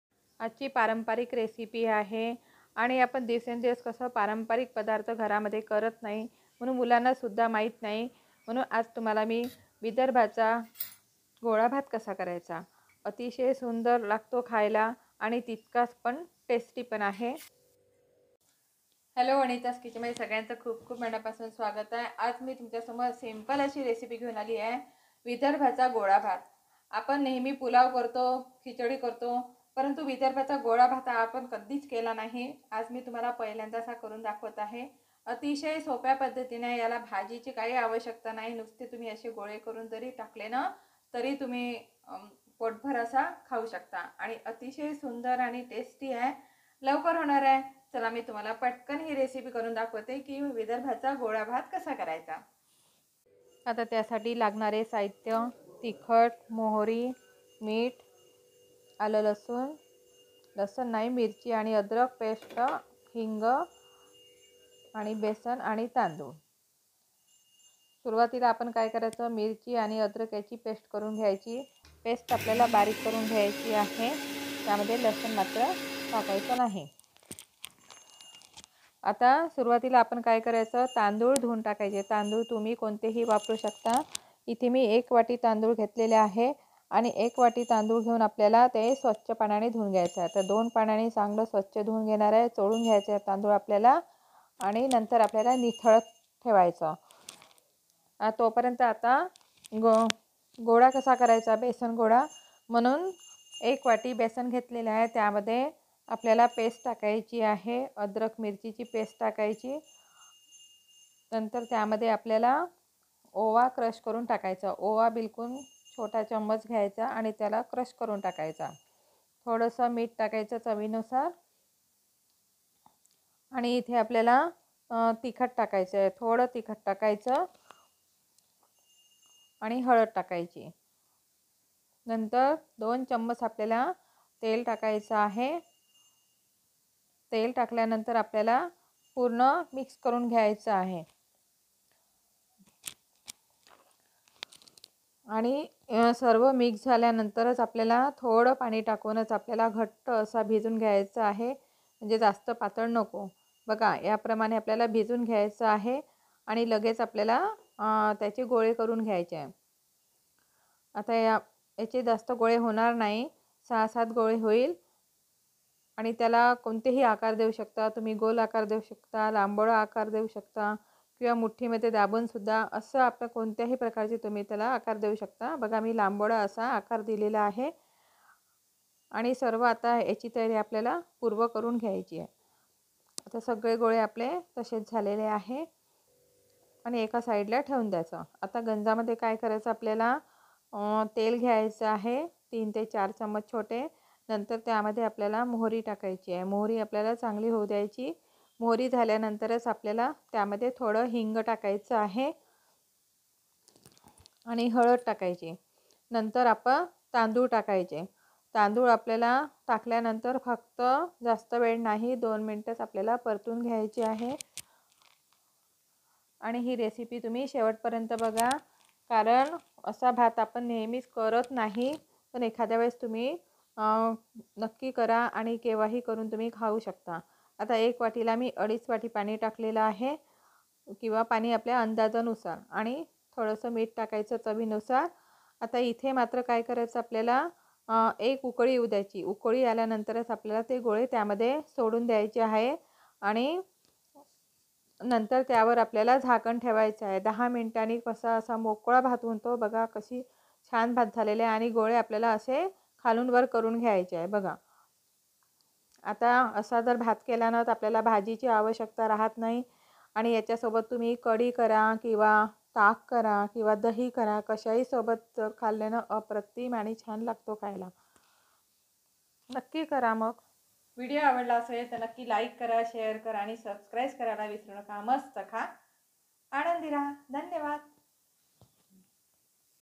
पारंपरिक! विदर्भाचा स्पेशल गोळाभात नागपूरचा गोळा भात!Vidharbhacha Golabhat! anitas gruhini kitchen
पारंपरिक recipe विदर्भाचा गोळाभात  कसा बनवायचा?
साहित्य
1वाटी तांदूळ
1वाटी बेसन
मिरची पेस्ट 1 चमचा
आले पेस्ट 1 चमचा
ओवा
हळद
तिखट
तेल
मिठ
कोथिंबीर
हिंग